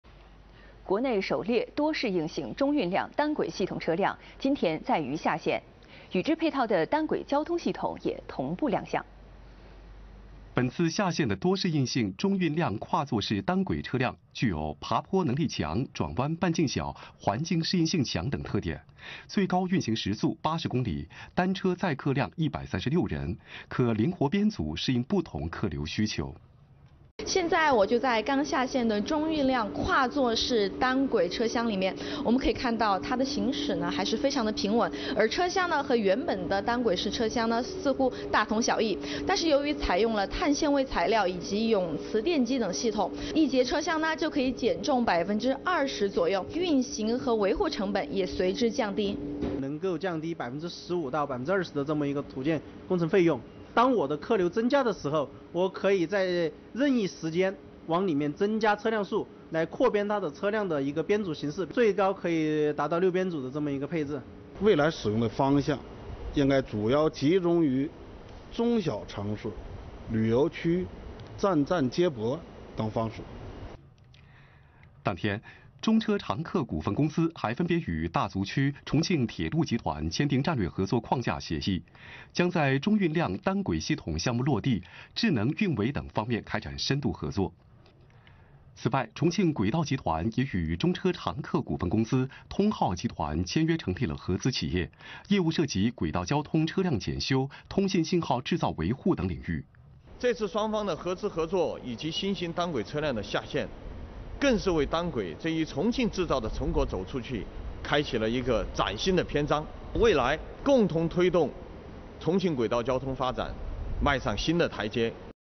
中国首列多适应性中运量单轨系统车辆在重庆下线
2月10日，国内首列多适应性中运量单轨系统车辆在两江新区重庆中车长客轨道车辆有限公司厂区下线。
相较于传统单轨，本次下线的车辆空车重量减轻约20%，可进行2-6节灵活编组，且爬坡能力强，适应高温高寒等环境。该车辆每节车厢内设有14个固定座位和4个可折叠座位，单车载客量为136人，每小时可以运送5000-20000人，最高运行速度为每小时80公里。该车辆有望率先在大足区投入使用。